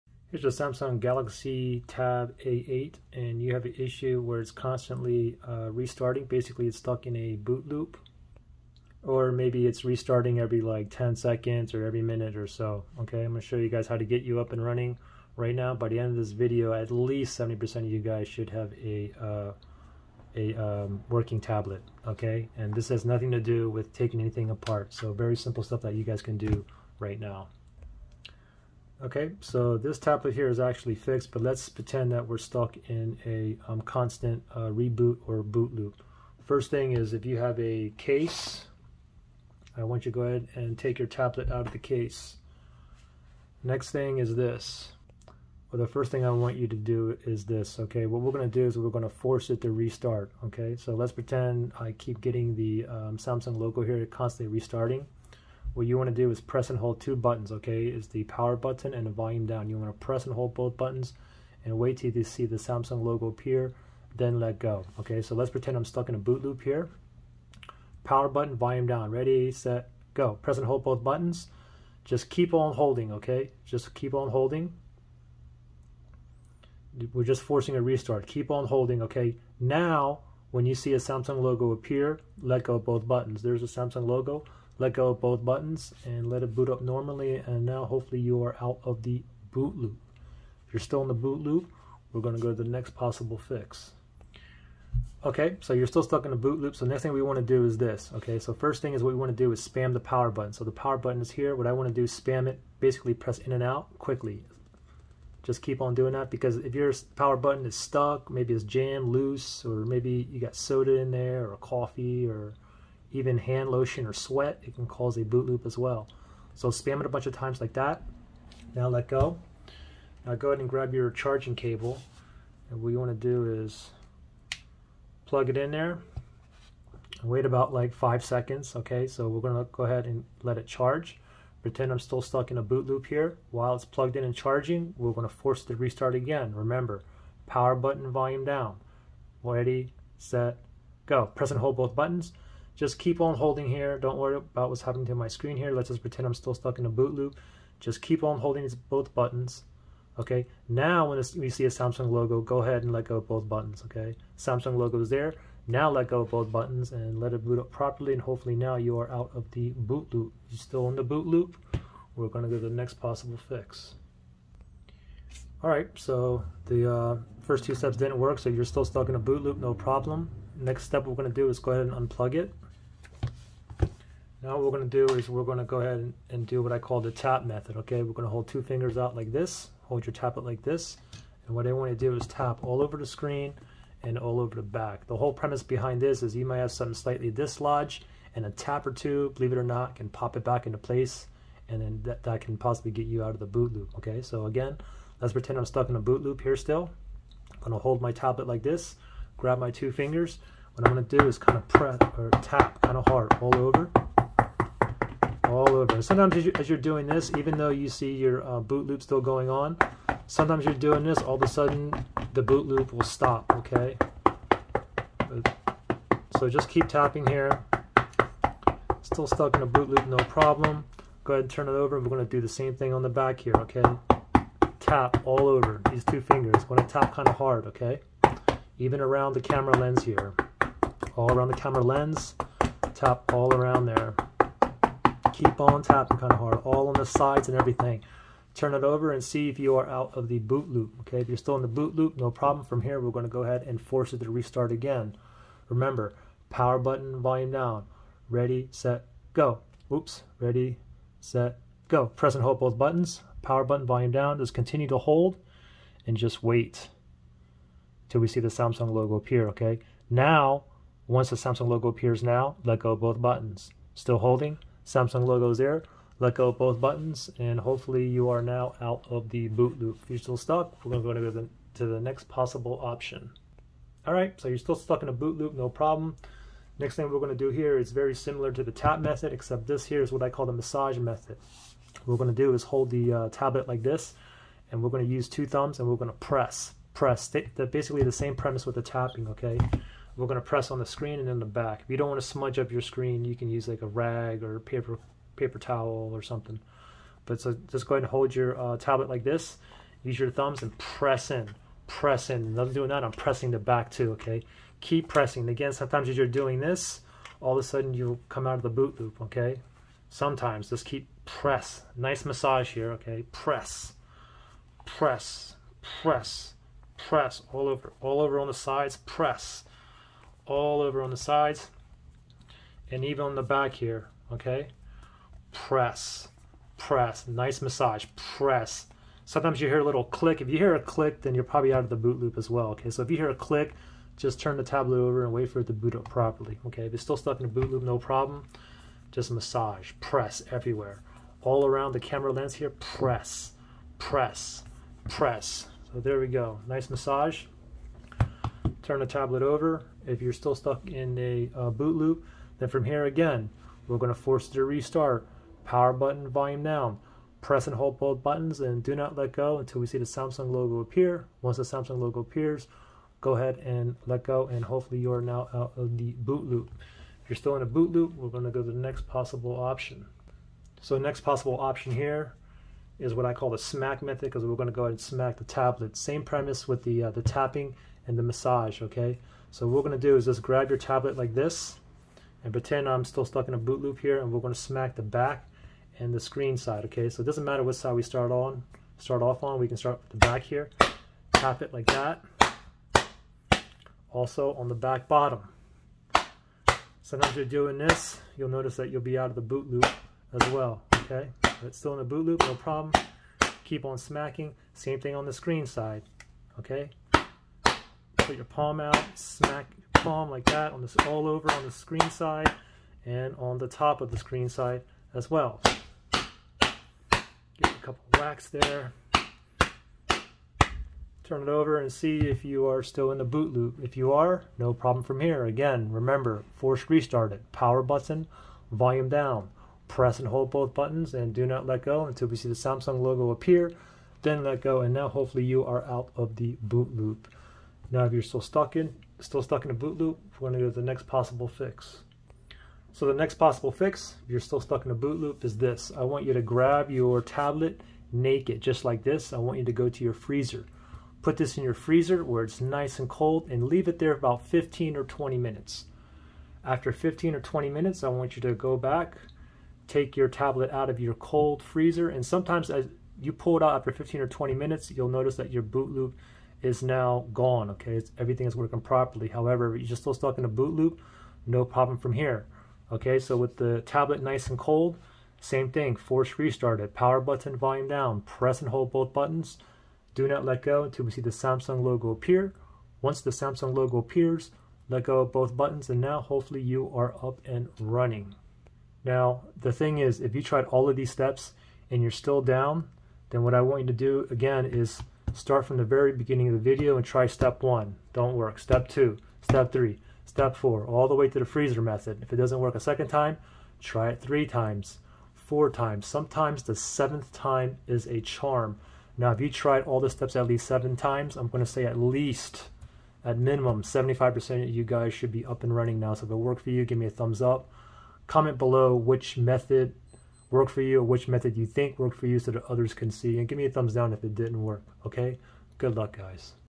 Galaxy Tab A8: Constantly Restarting? Stuck in Boot Loop? Easy Fixes!
I show you how to fix a Samsung Galaxy Tab A8 that is stuck in a boot loop where the Tab continuously restarts over and over again. Plz comment which step worked for you so other can see. Hope this helps.

Samsung Galaxy Tab A8 Android Tablet, 10.5” LCD Screen